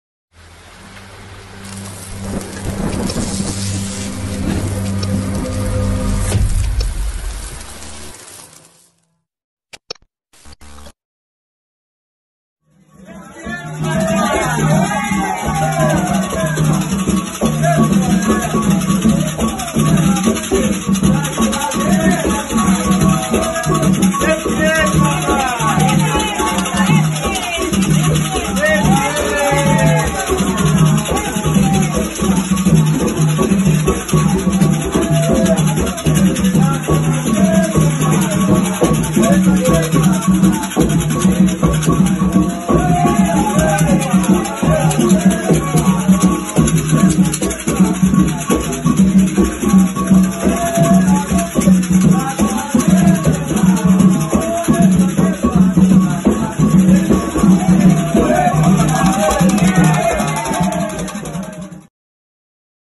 SAÍDA DO MUZENZA GANGA MUXICONGO DE KASSUTÉ - ABAÇA INKOSSI TALADECY
MAIS UM MUZENZA QUE NASCE NA NAÇÃO ANGOLA,TOROJITALA DE INKOSSI (PAI HÉRCULES D'OGÚN)